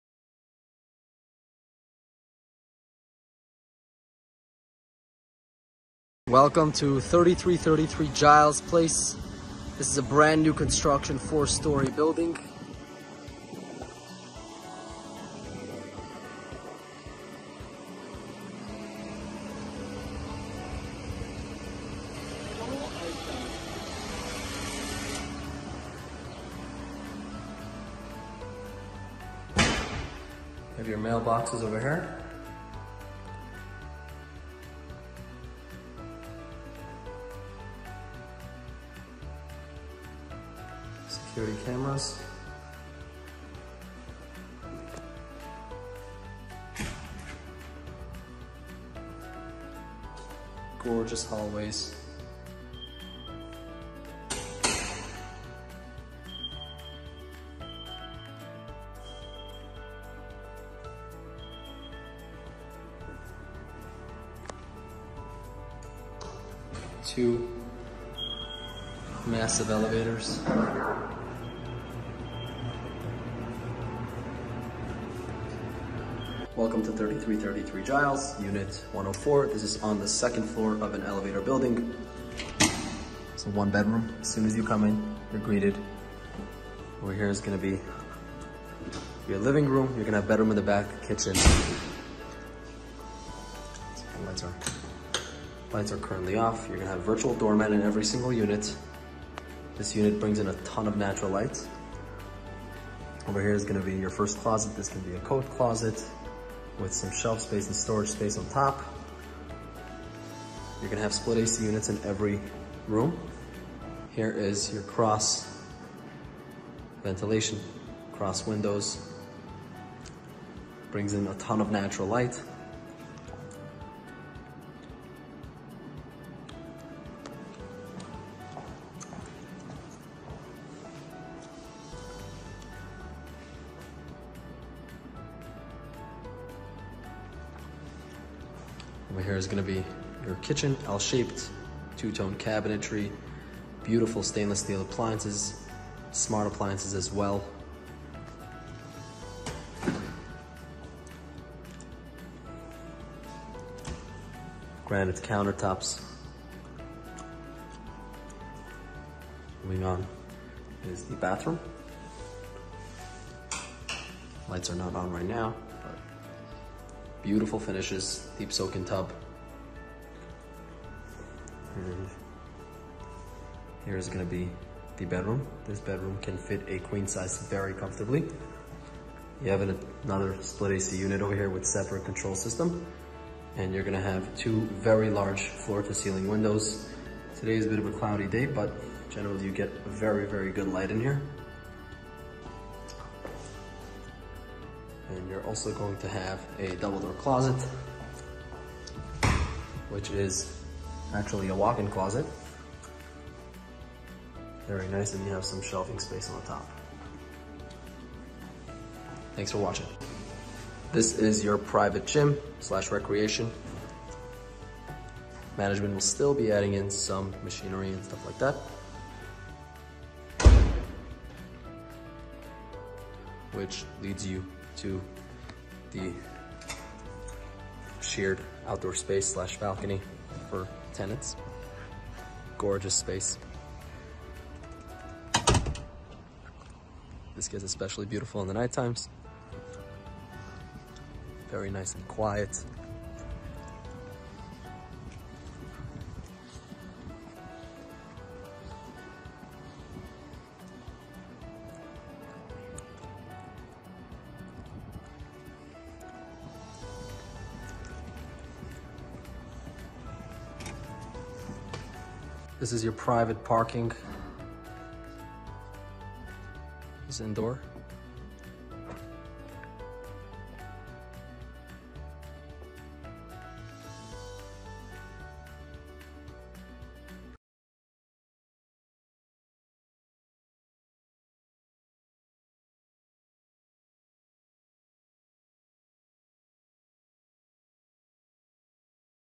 3333 Giles Place #104 (1 Bedroom)
Beautiful 1-bedroom apartment in Kingsbridge Heights, Bronx.